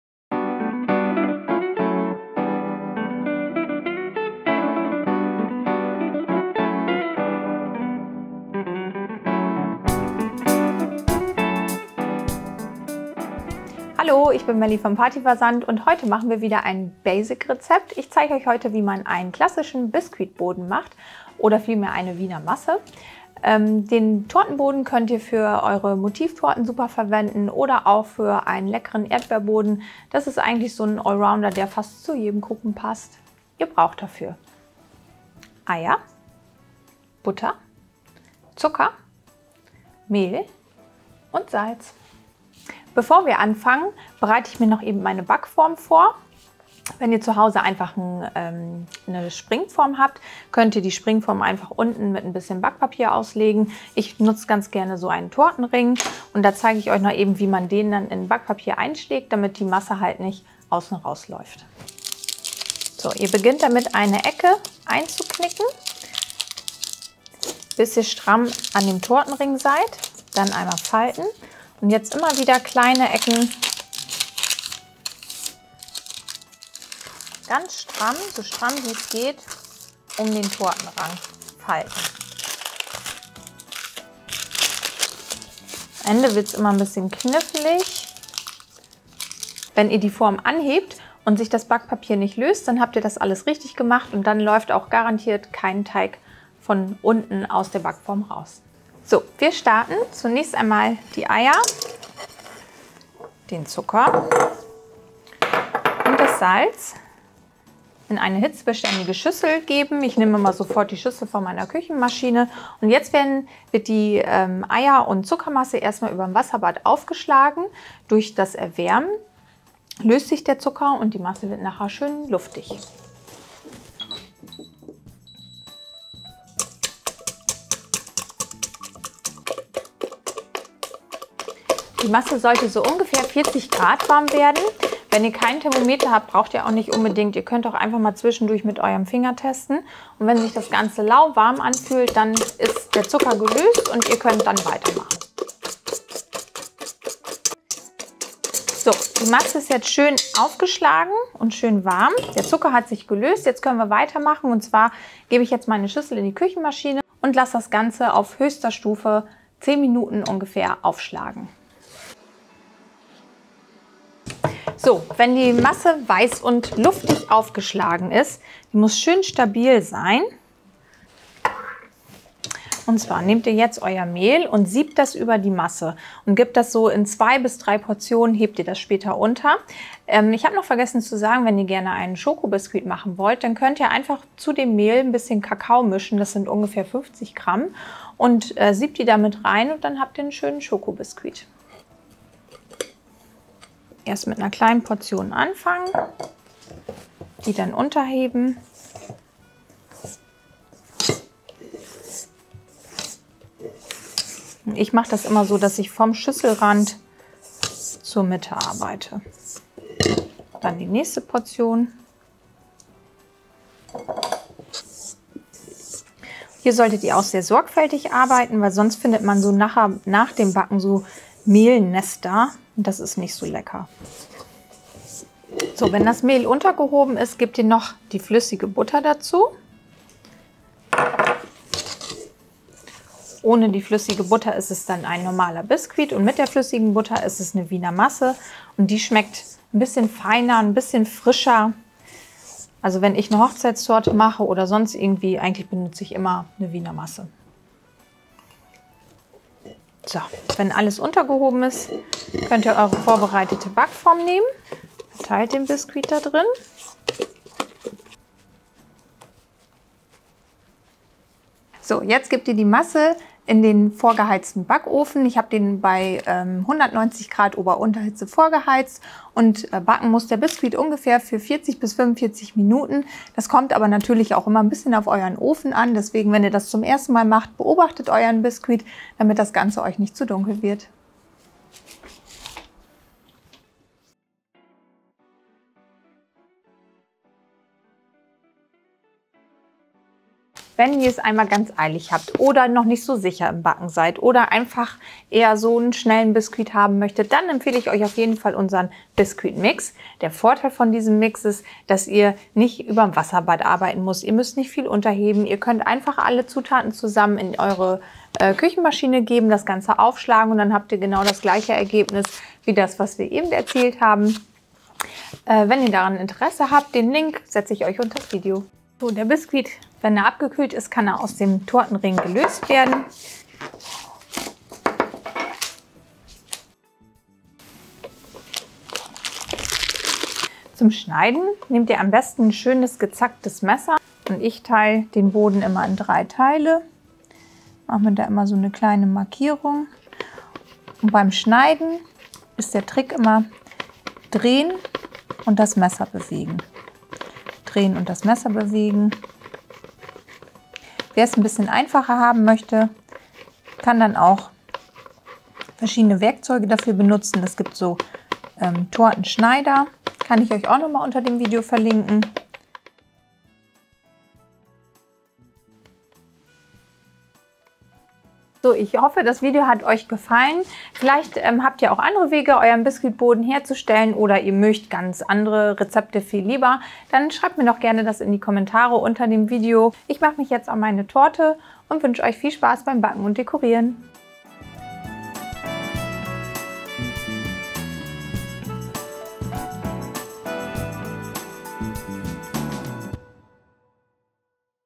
Biskuitboden 🤎 Grundrezept für eine Wiener Masse
Der Boden ist die ideale Basis für Eure Kuchen und Torten! Diesen lockeren und fluffigen Biskuit könnt Ihr ganz einfach nach dem Backen in drei Teile schneiden. Damit habt Ihr die perfekte Grundlage für Eure nächsten Traumtorten! In diesem Video zeigen wir Euch ein einfaches Grundrezept für einen fluffigen Biskuitboden.

Zutaten für einen Tortenboden mit ca. 26cm Durchmesser
300g Vollei (6 Eier Gr. M)
150g Zucker
1 Prise Salz
180g Mehl
50g Butter (flüssig)

Für die Kuchen die Eier, Zucker und Salz in eine hitzebeständige Schüssel geben und mit einem Schneebesen über dem Wasserbad warm schlagen (ca. 40°C). Die Masse anschließend in der Küchenmaschine oder mit einem elektrischen Handrührgerät ca. 10 Min. auf höchster Stufe hell und cremig aufschlagen. Das Mehl über die Masse sieben und vorsichtig unterheben. Die flüssige Butter dazugeben und vorsichtig unterrühren. Hierbei nicht zu lange rühren. Die Masse in eine mit Backpapier ausgelegte Backform bzw. einen Tortenring füllen und im vorgeheizten Backofen bei 190°C Ober-/Unterhitze ca. 40 Min. backen, anschließend auskühlen lassen.